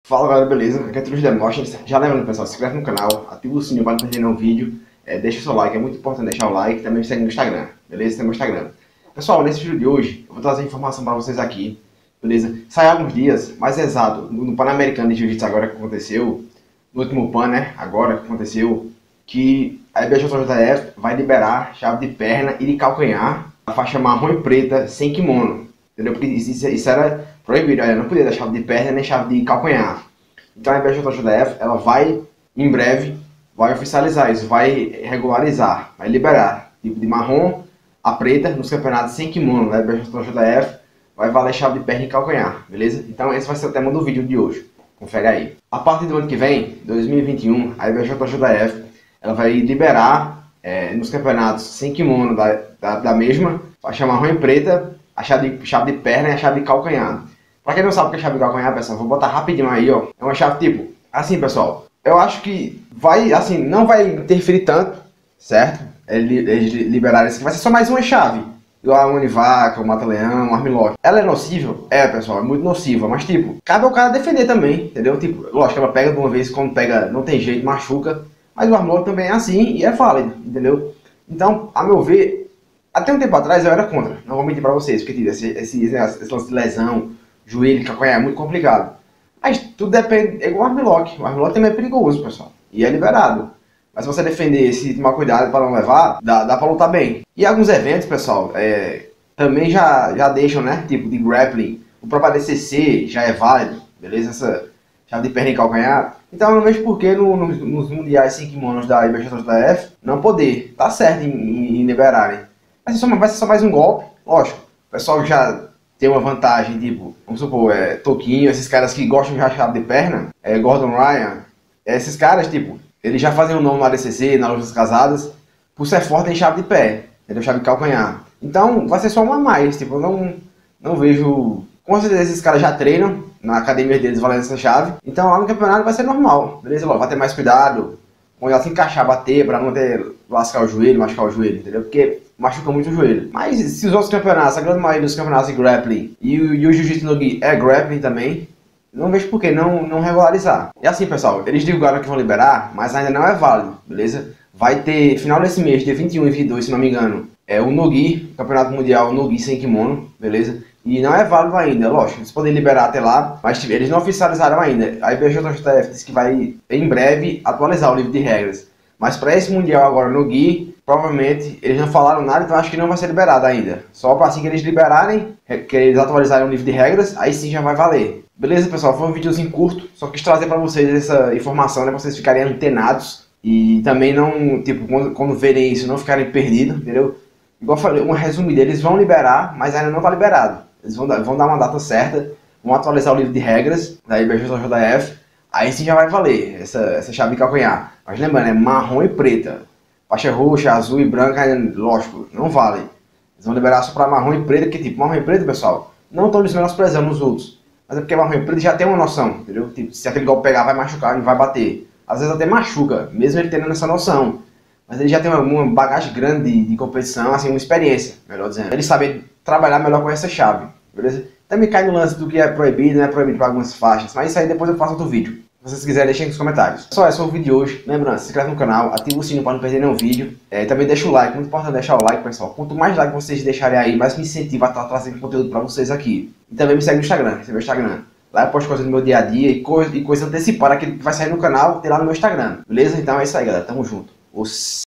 Chave de CALCANHAR LIBERADA NA IBJJF (Heel Hook)
Novidade para vocês, a partir de 2021 estará liberada a chave de calcanhar nos eventos Sem Kimono da IBJJF!! No vídeo a cima explico melhor